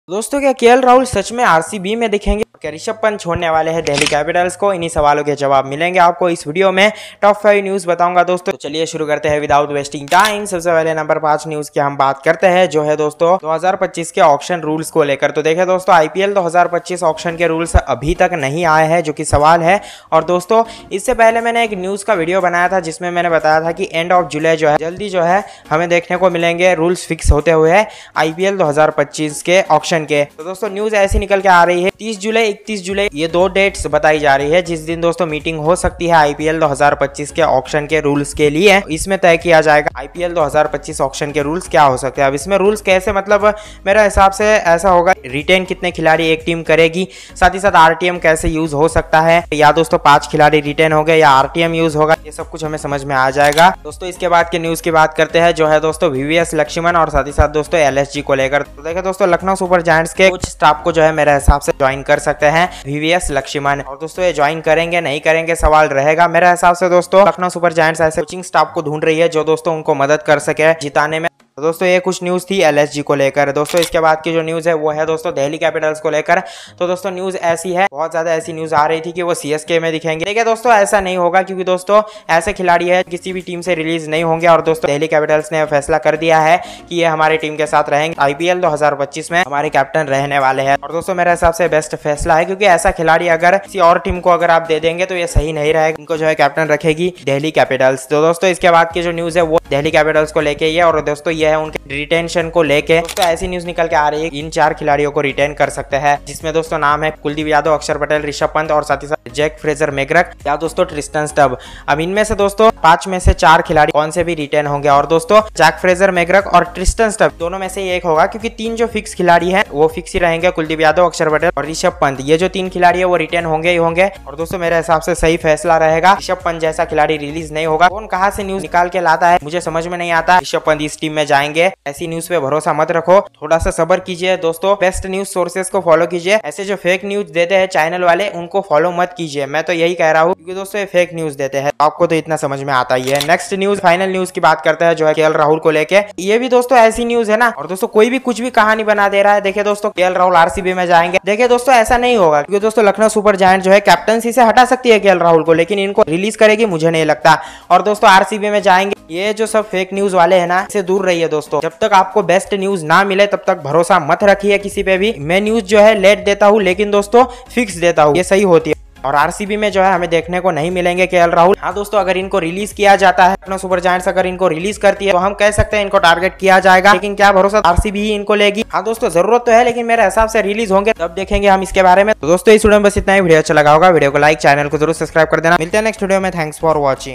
IPL 2025 TOP 5 News || IPL 2025 Auction Rules Fix Date , Kl Rahul in RCB 😒
In This Video We are discuss About a IPL 2025 News

00:10 Auction rules Is Fix On 31-30 July

00:53 VVS Laxman Likely Join LSG

01:13 Rishabh Pant is Captain of DC in IPL 2025

01:43 DC likely Retain 4 Players

02:23 KL Rahul Is Not in RCB